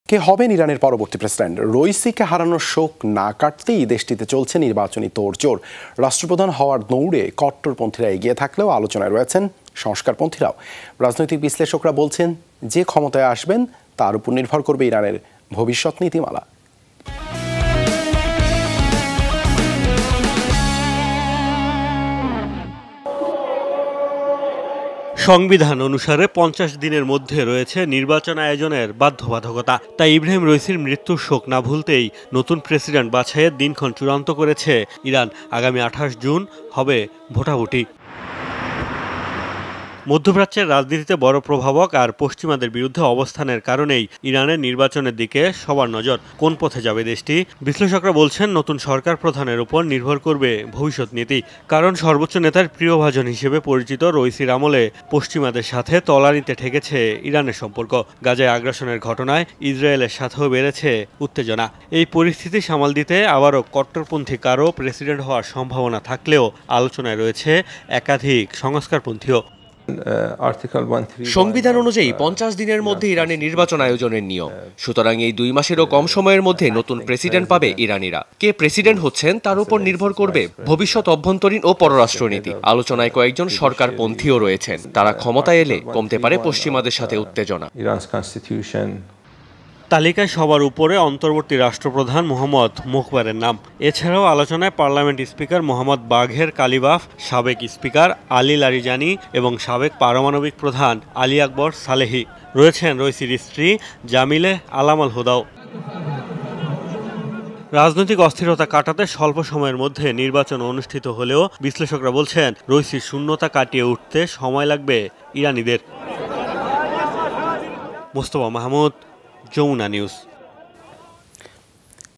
ইরানের পরবর্তী প্রেসিডেন্ট হওয়ার দৌড়ে এগিয়ে যারা | Iran New President| Jamuna TV
কে হবেন ইরানের পরবর্তী প্রেসিডেন্ট? - রইসিকে হারানোর শোক না কাটতেই দেশটিতে চলছে নির্বাচনী তোড়জোড়। রাষ্ট্রপ্রধান হওয়ার দৌঁড়ে কট্টরপন্থিরা এগিয়ে থাকলেও; আলোচনায় রয়েছেন সংস্কারপন্থিরাও। রাজনৈতিক বিশ্লেষকরা বলছেন, যে ক্ষমতায় আসবেন তার ওপর নির্ভর করবে ইরানের ভবিষ্যৎ নীতিমালা।

Address:
Bangladesh.